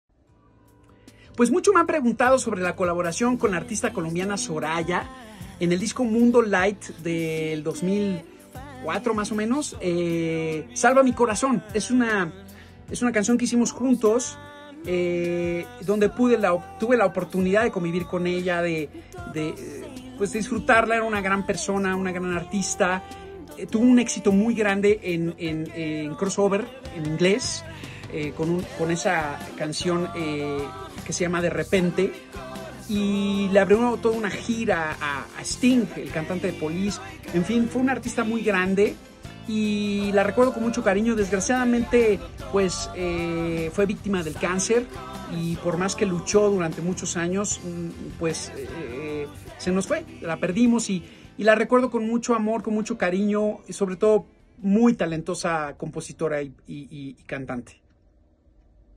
Mi colaboración con Soraya
Recordando con mucho cariño a la cantante Soraya quién cumpliría años el pasado 11 de marzo. ¡Un abrazo hasta el cielo!.. Hoy les cuento un poco de mi colaboración con ésta talentosa mujer... ⭐🙌🎶